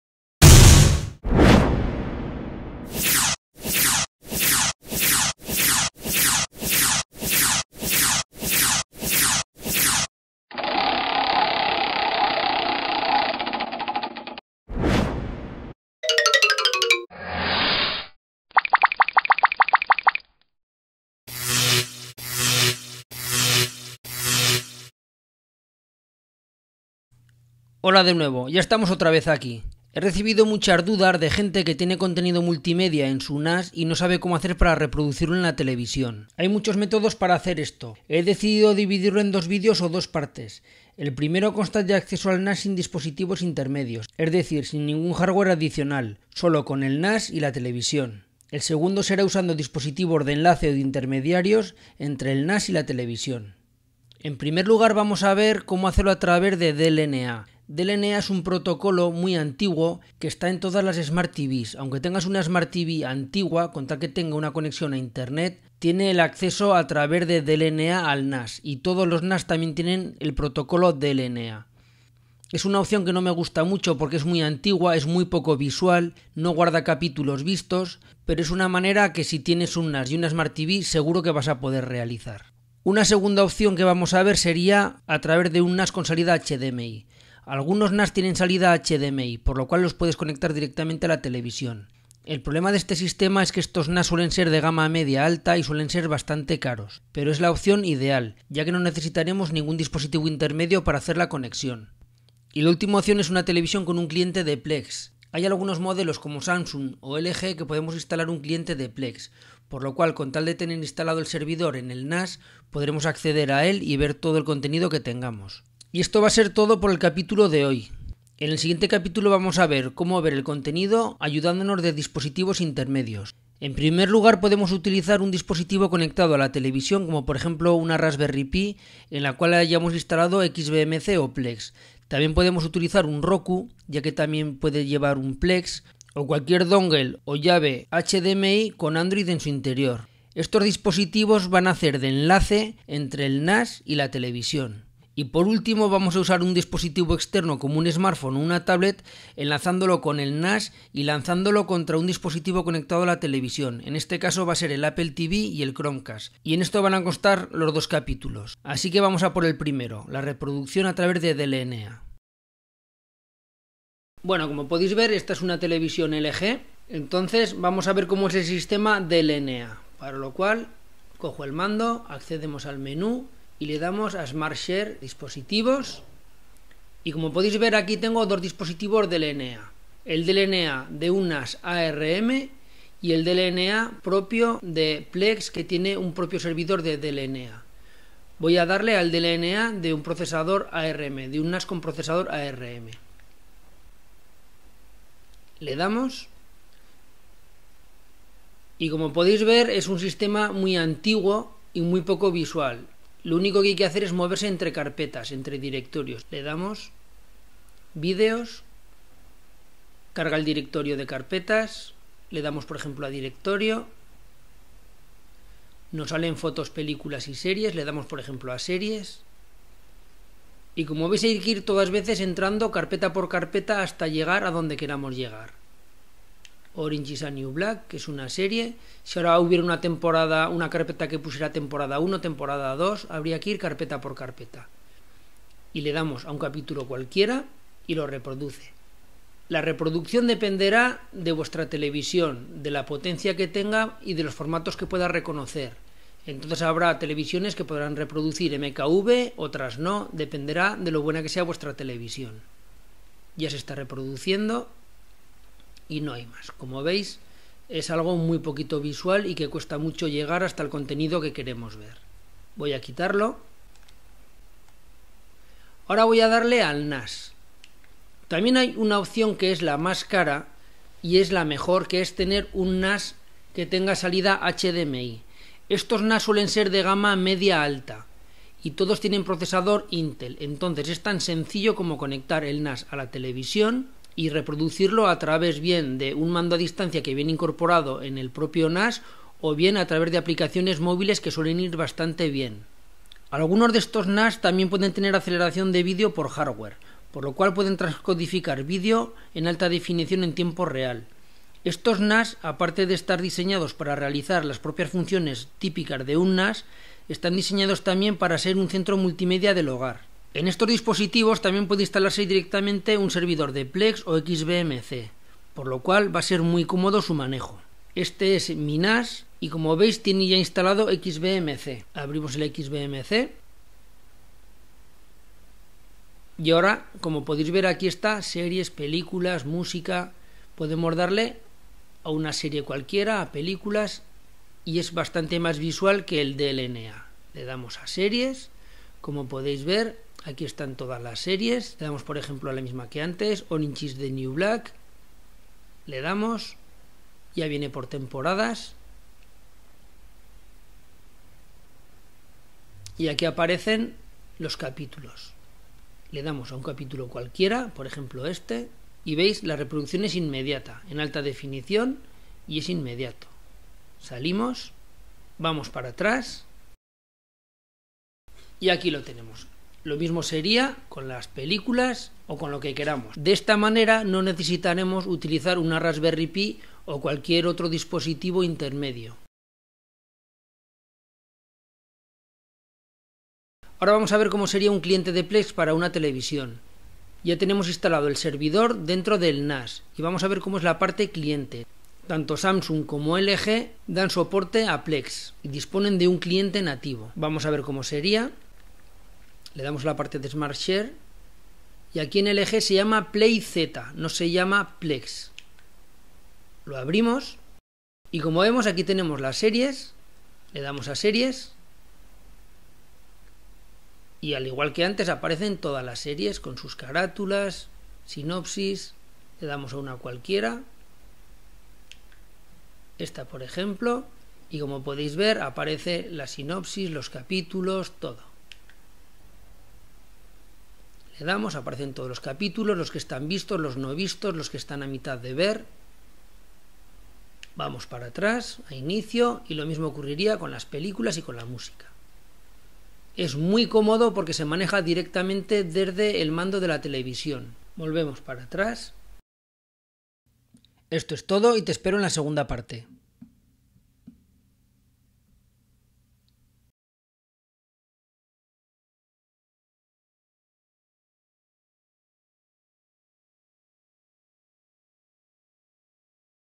Cómo ver el contenido de tu NAS en la televisión (parte I)
La opción más sencilla es utilizar un NAS con salida HDMI y conectarlo directamente a la televisión. Es muy cómodo, ya que podemos controlarlo con el mando a distancia del propio NAS o con alguna aplicación para smartphone.
Una solución mucho más barata es utilizar el protocolo DLNA. Todas las smartTV con acceso a internet, por muy antiguas y obsoletas que sean, disponen de acceso DLNA. Este protocolo de comunicaciones también está presente en todos NAS, por lo que siempre vamos a poder acceder a nuestros contenidos mediante este sistema.
Finalmente si tenemos una smartTV de Samsung o LG podemos instalar un cliente de Plex, ya que estas dos marcas dan soporte a este mediacenter. Es tan sencillo como instalar el servidor de Plex en el NAS, el cliente en el televisor.Recuerda que en LG la aplicación se llama “PLAYz” y no Plex.
Si nuestra televisión no goza de acceso internet o simplemente no te gustan estas soluciones podemos recurrir a una gran variedad de recursos para ampliar la capacidad de nuestro sistema multimedia. Debido a que el repertorio es muy amplio lo dejo para una segunda parte donde te explicaré de manera detallada todas las soluciones disponibles en el mercado.